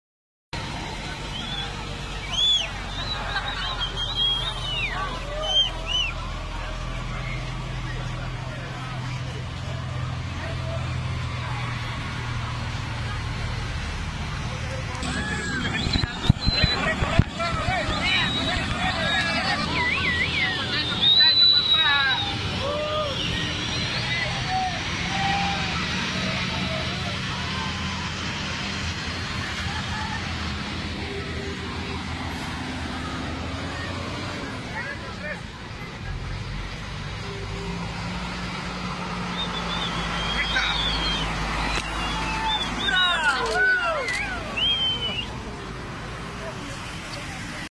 HONDURAS Montados sobre rastras y camiones hondureños huyen de la POBREZA
HONDURAS Continua el éxodo.